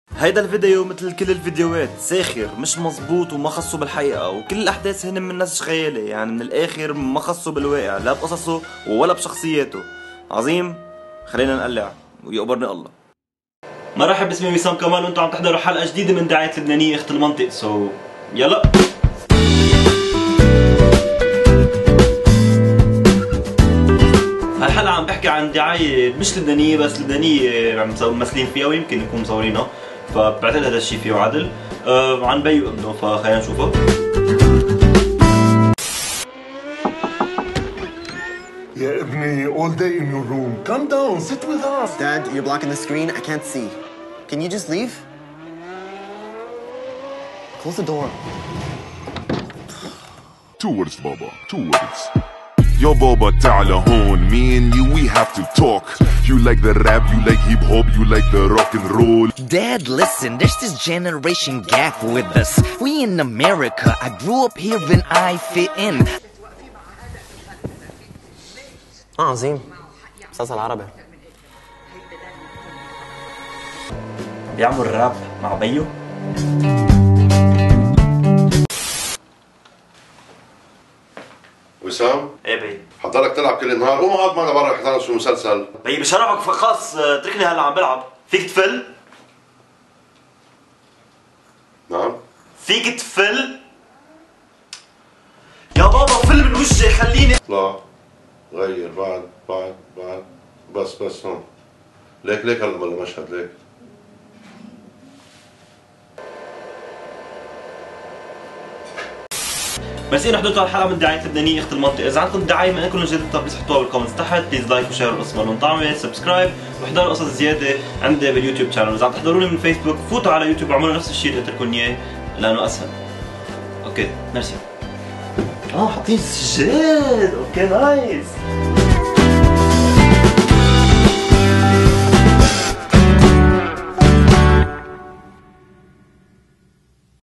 Rapping Dad | Lebanese Ads: F**k Logic
Ads, we all hate them. Well some of them, simply because they're illogical.

Rapping Father and Son.

PS: I do not own the original Ad shown footage.